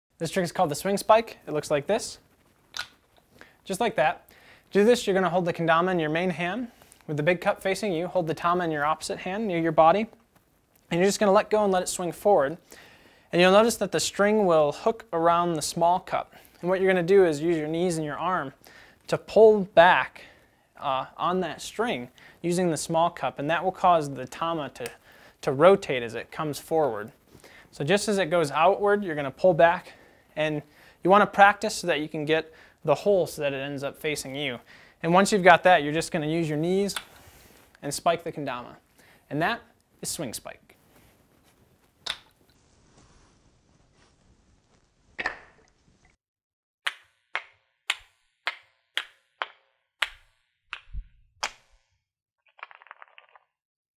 Learn the Swing Spike Kendama Trick
Learn how to do the Swing Spike Kendama Trick. Featuring the LK Standard Kendama.
The Swing Spike is a variation of the Pull Spike that has a clever use of the string and small cup to land the spike.